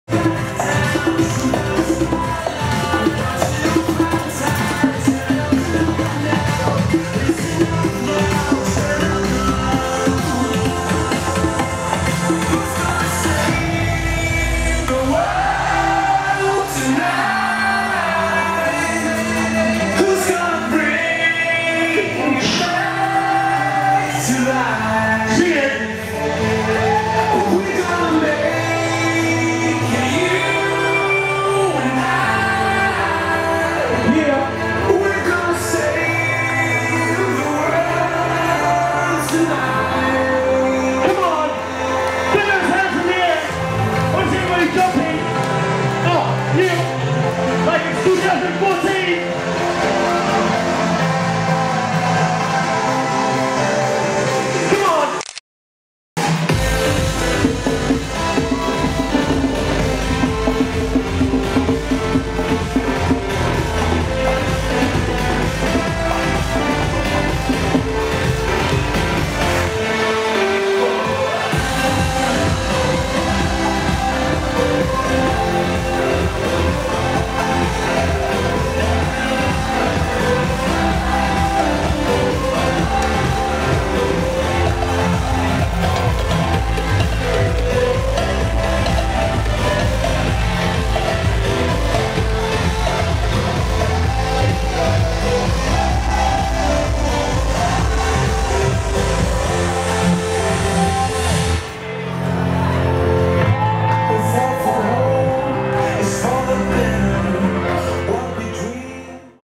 Dekdrum Percussion live at NYE h2oh feat. Vamp
Dekdrum Interactive Percussionist and performer plays custom congas, bongos and cowbell live at New Years Eve H2oh event in front of a full house at Showtime Entertainment Centre. Dekdrum plays bongos and percussion live alongside DJs.